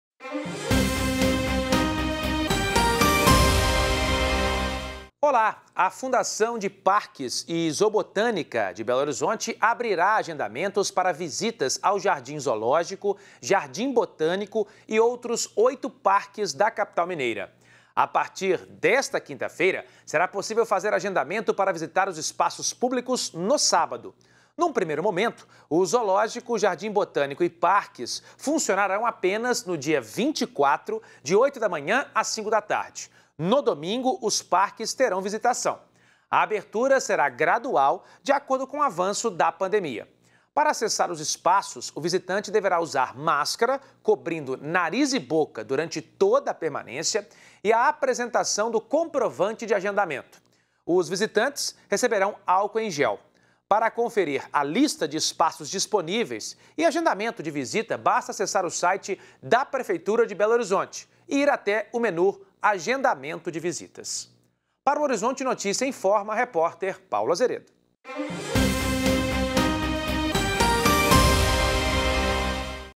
Horizonte Notícia Informa: PBH abre agendamentos para visitas aos parques | 22/04/2021 – Edição 03
A Fundação de Parques e Zoobotânica abrirá agendamentos para visitas ao Jardim Zoológico, Jardim Botânico e outros oito parques de Belo Horizonte. A partir desta quinta-feira será possível fazer o agendamento para visitar os espaços públicos no sábado.Mais detalhes com o repórter Paulo Azeredo.